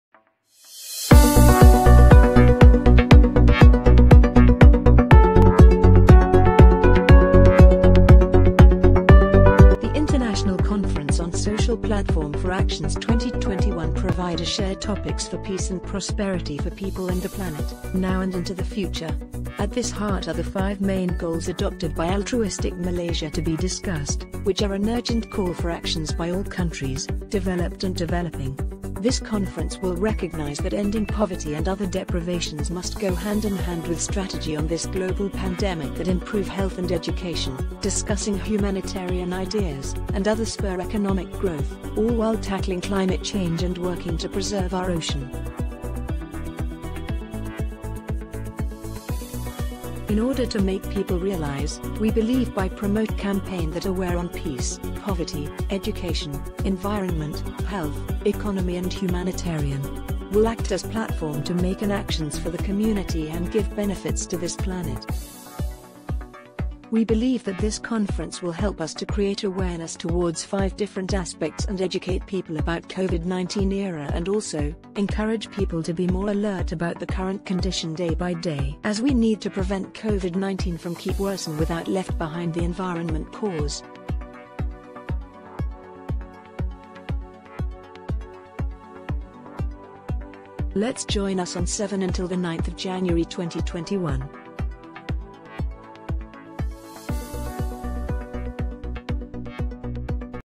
INTERNATIONAL CONFERENCE ON SOCIAL PLATFORM FOR ACTIONS (ICSPA) 2021
Registration for participants open now! The conference discussing the current world issue with experts, change makers, activists, youths, entrepreneurs and innovators over the world and addressing the solutions together will be the best experience in the International Conference on Social Platform for Actions 2021. You are able to share your idea for a better future world.

ICSPA 2021 will become a platform that connects you with varies background professional from all over the world.

By joining ICSPA 2021, you will get a certificate that is recognized at International Participations and this is free entrance conference with the motive to wide the knowledge broaden.